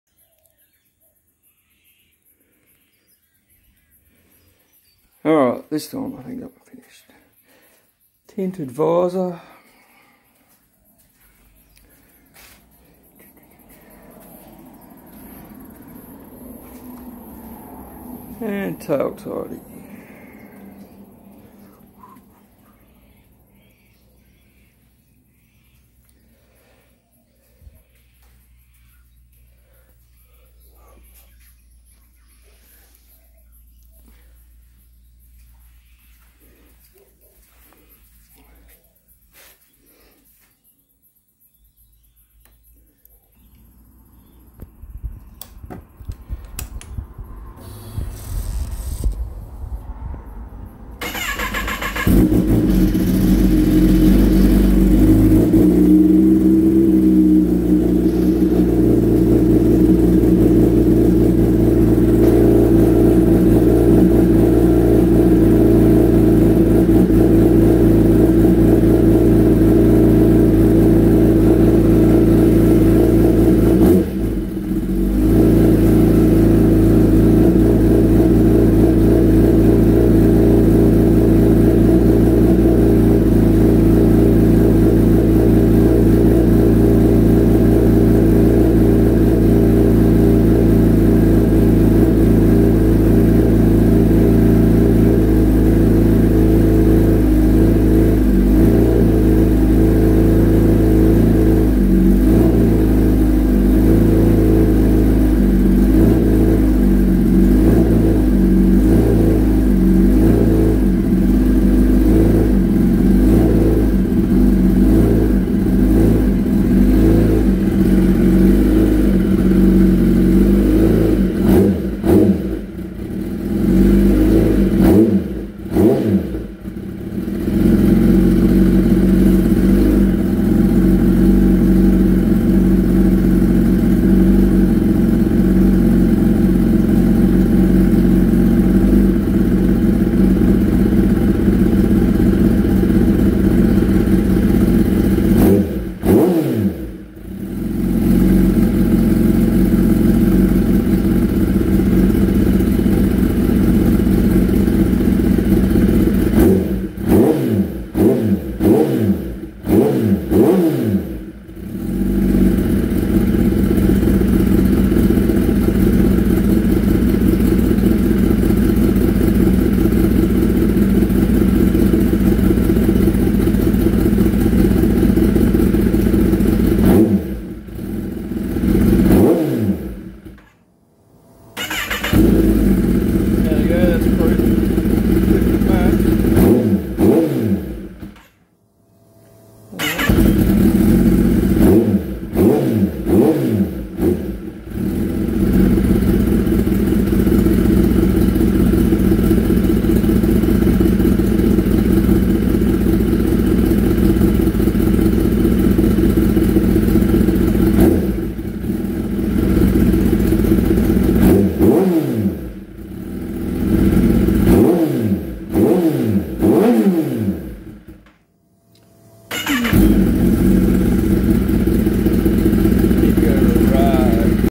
2016 Kawasaki ZX10R KRT Edition - FINISHED PART 2
Your never really finished are you!

OK, this update was a tail tidy and a black screen which is a tiny bit higher but not much if any.  The screen is 4 screws and about a minute with a cordless drill.  Tail tidy uses factory indicators, I am not a fan of the tiny LED ones. I have a few sets I have tried on my Honda SP1 and they are absolutely  pathetic in daytime. They just aren't bright enough to be safe.

I also updated my 3 maps on the ECU using Woolich Racing software.
Full = Kawasaki factory map
Medium = Mapshare dyno map
Low = Mapshare autotune map

Found out from Woolich it's the intake air pressure or IAP fuel map that was making it stall.  Copy and paste the Factory IAP map over and it's perfect.

I will make a new video when I remember.  My other 2 bikes have been dynoed and I know that's the way to go.  Just having a bit of DIY fun with Woolich to learn more about it.

Cheers

Ash